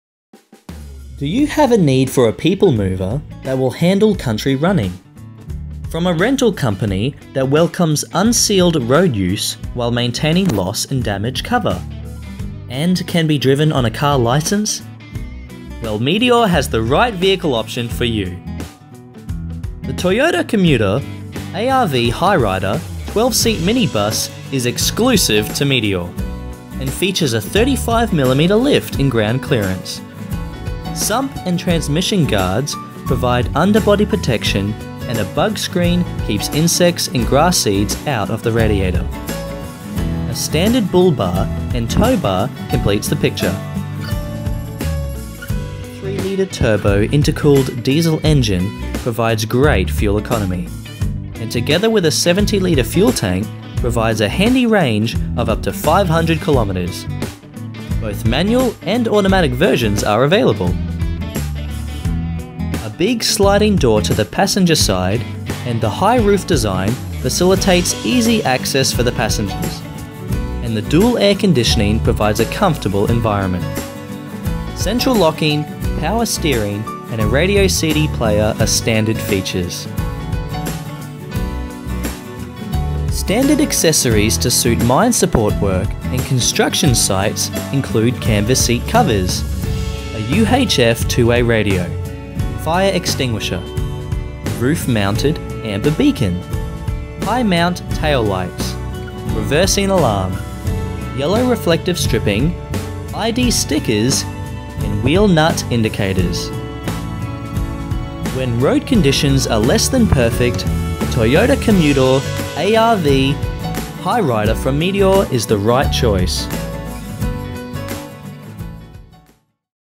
U1950 Toyota Commuter Townsville Mackay Mt Isa Cairns
Meteor car rental car hire & Truck Rental

U1950 Toyota Commutor ARV 12 seat minibus - Automatic
Do you have a need for a people mover that will handle country running? From a rental company that
welcomes unsealed road use while maintaining Loss and Damage cover? And can be driven on a car
licence? Meteor has the right vehicle option for you!

The Toyota Commutor ARV Hi – Rider 12 seat minibus is exclusive to Meteor and features a 35mm lift
in ground clearance. Sump and transmission guards provide under body protection and a bug screen
keeps insects and grass seeds out of the radiator. A standard bull bar and tow bar completes the
picture!

A powerful 3 litre turbo intercooled diesel engine provides great fuel economy, and together with a 70 litre
fuel tank provides a handy range of up to 500 kilometres. Both manual and automatic versions are available.

A big sliding door to the passenger side and the high roof design facilitates easy access for the
passengers, and the dual air conditioning provides a comfortable environment. Central locking, power
steering and a radio CD player are standard features.